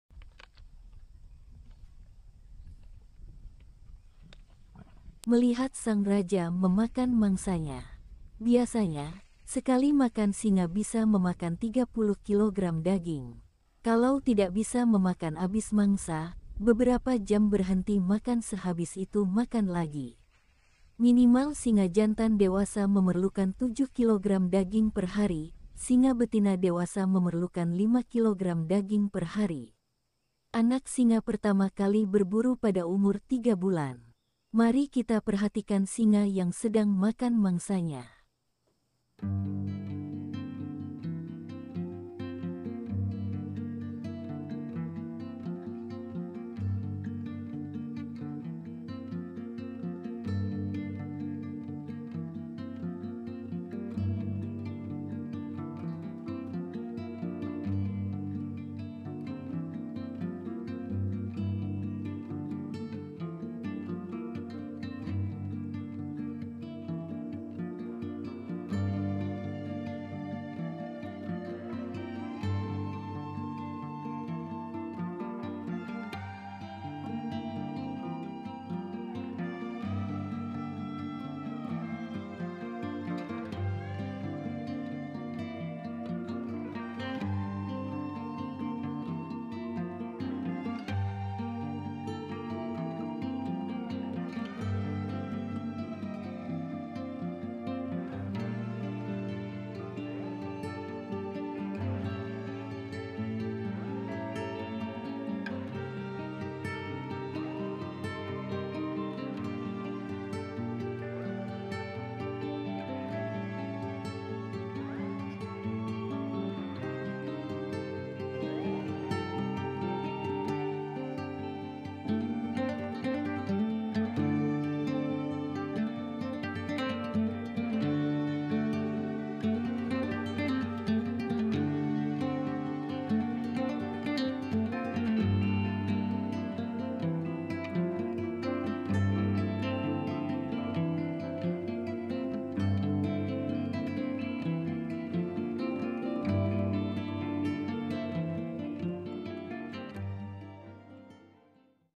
Sang Raja Sedang Berpesta (Singa Kelaparan) Berapa Banyak singa Bisa makan.?
Diketahui hingga sekarang ini ada sekitar 30.000 hingga 100.000 singa yang tersisa di Bumi. Dalam beberapa dekade terakhir, populasi singa telah menurun hampir setengahnya. Perburuan trofi dan hilangnya habitat alami adalah alasan utama berkurangnya populasi singa.